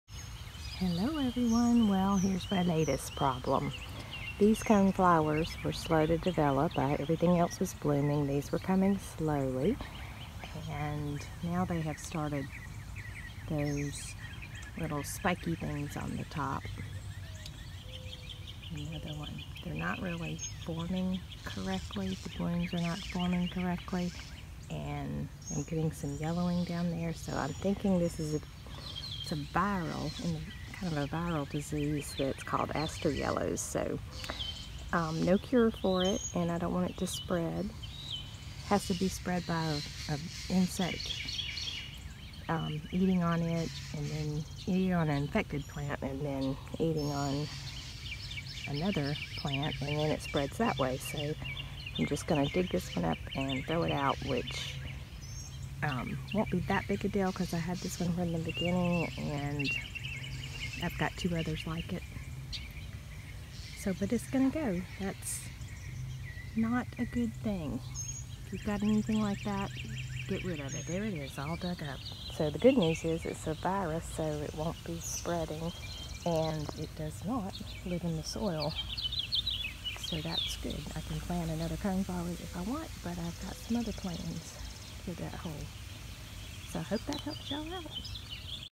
Aster Yellows???
In this video I will show you what I was pretty sure at the time was Aster Yellows  a phytoplasma disease of plants in the aster family.  I later discovered that there is a Coneflower Rosette Eriophyid Mite that causes a similar irregular growth pattern on coneflowers.  Evidently is hard for even seasoned gardeners to tell the difference so I am including this link that talks a little more about both.